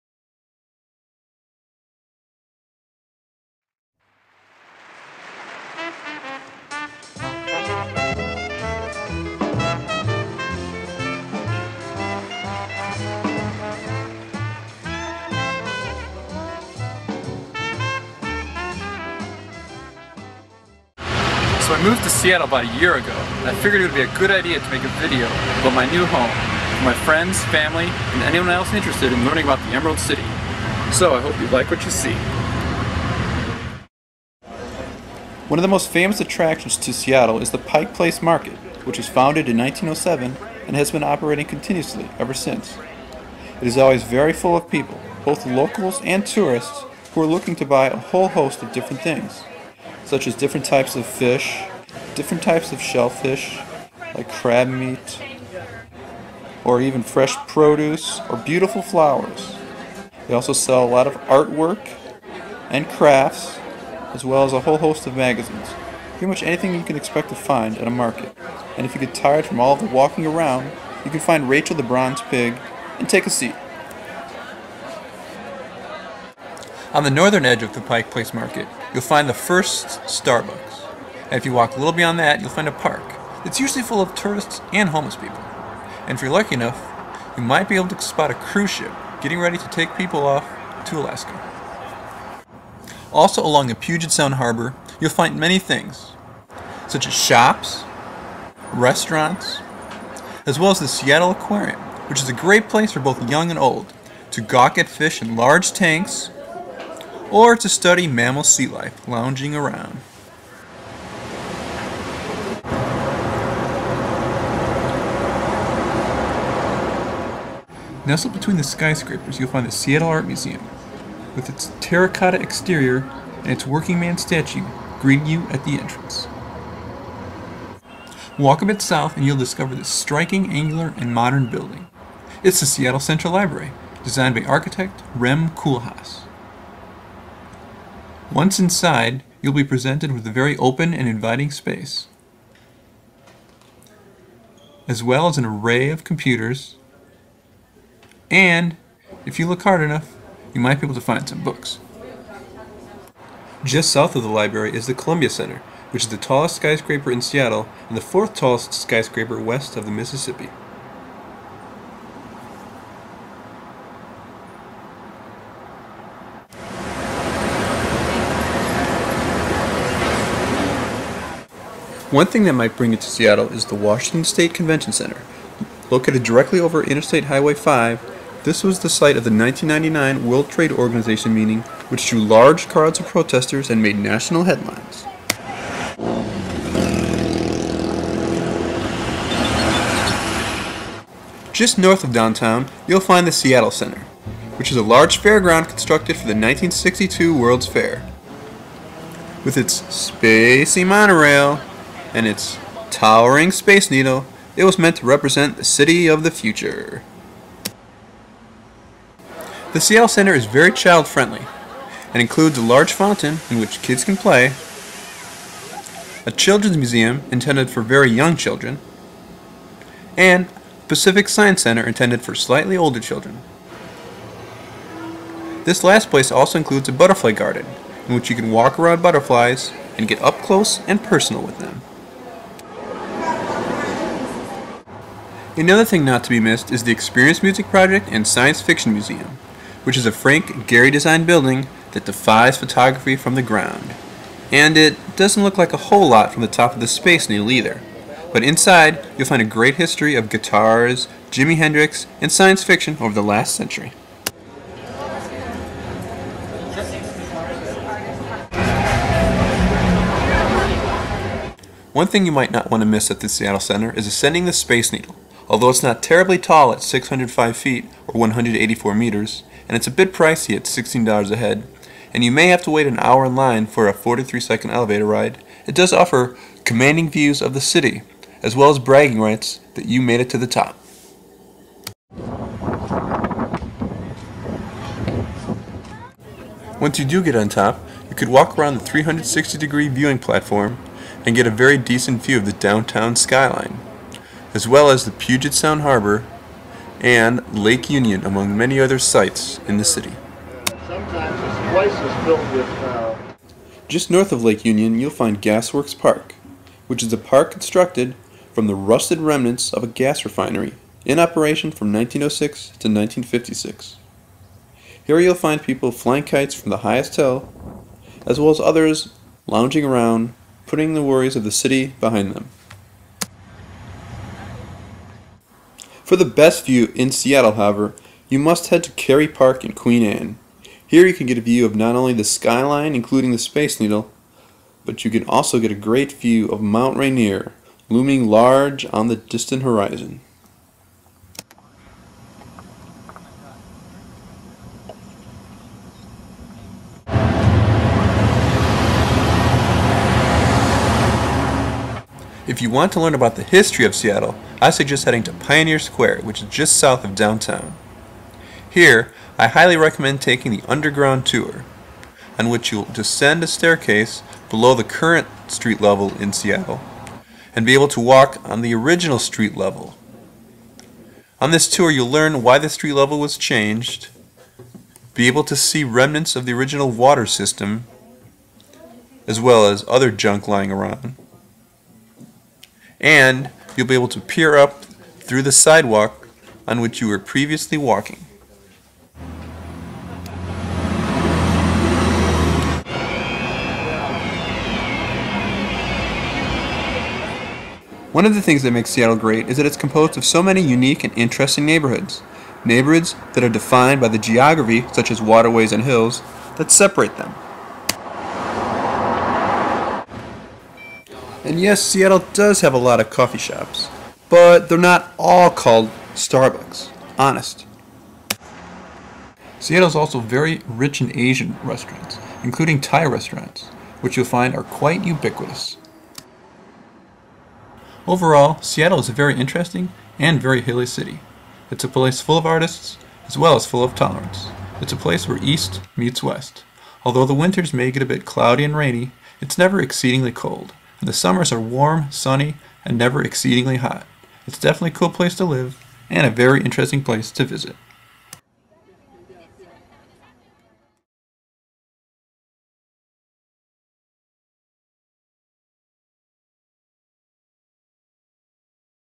Guide to Seattle
A guide to Seattle, Washington, filmed mostly in the summer and fall of 2008.